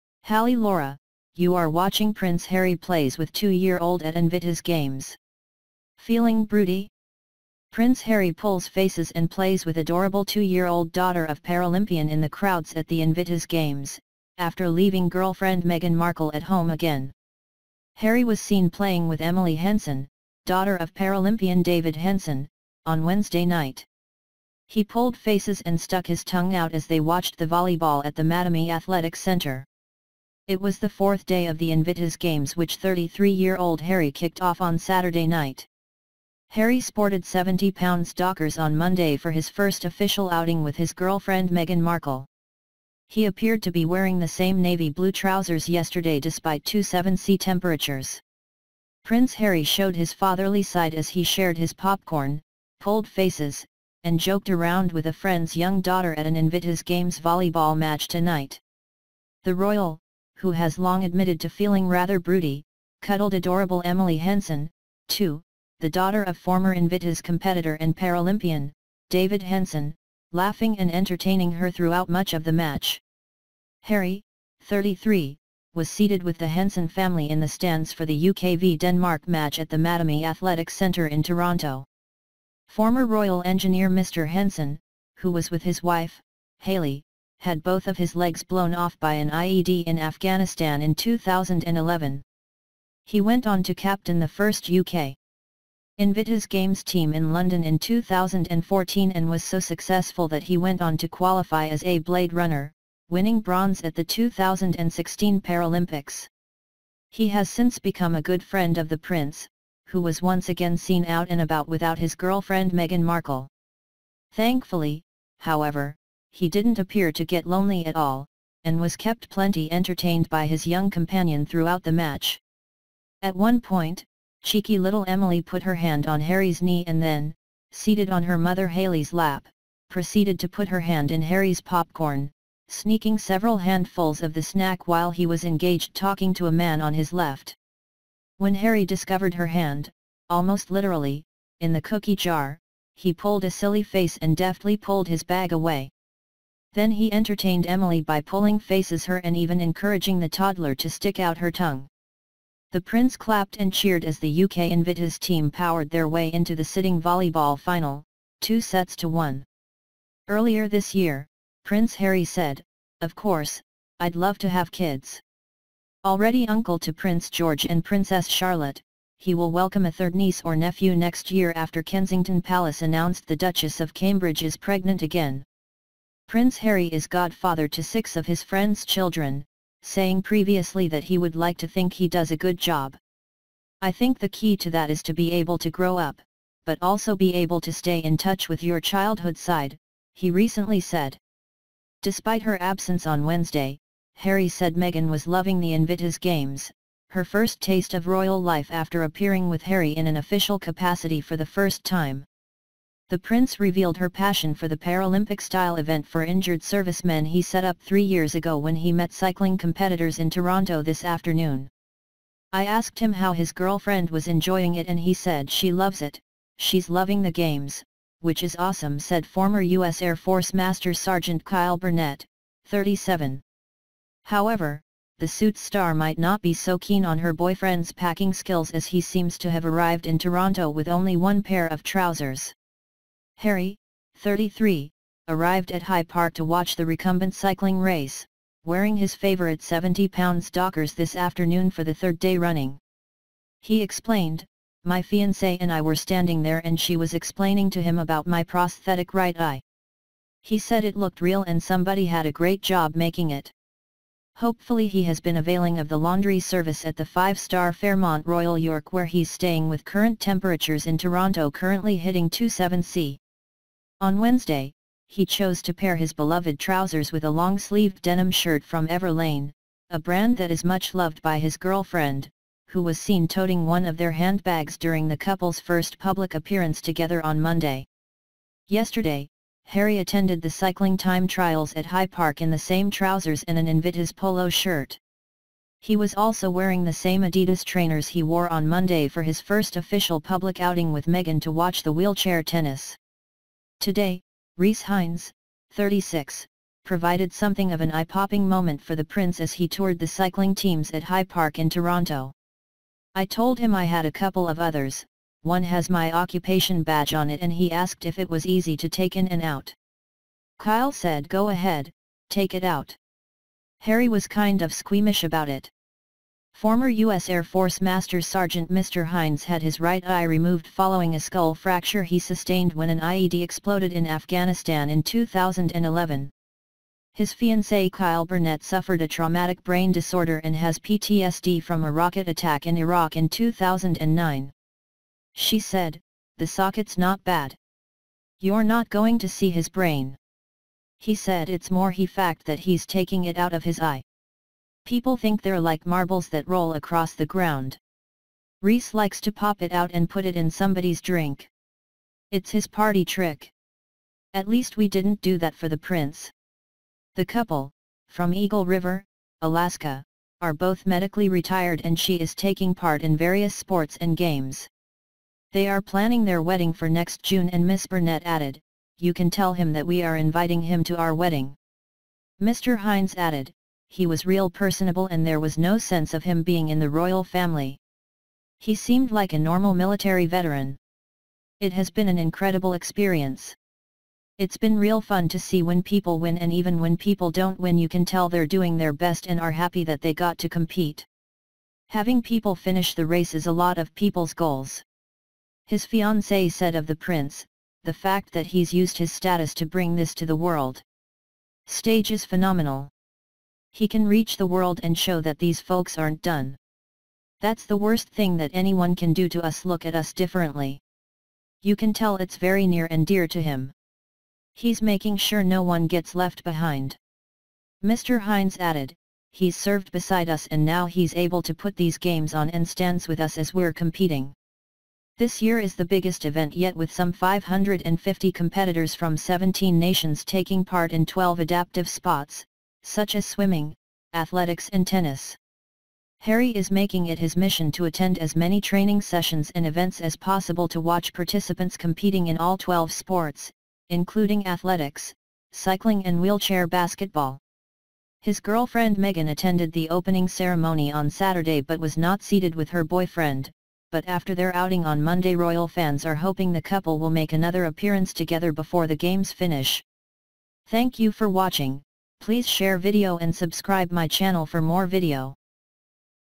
NewsRoyal - Prince Harry plays with two year old at Invictus Games
Feeling broody? Prince Harry pulls faces and plays with adorable two year old daughter of Paralympian in the crowds at the Invictus Games - after leaving girlfriend Meghan Markle at home again.